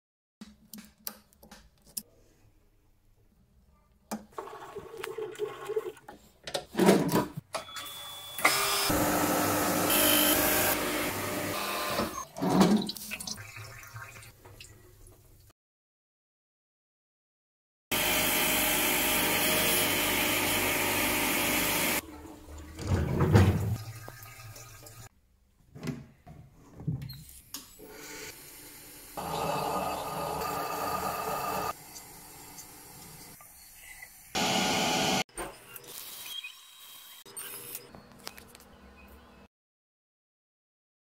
Sample preparation for Vickers hardness test ( threaded rod)
How to prepare sample with parallel sides for Vickers hardness test. Specimen is a threaded rod, ISO grade 4.8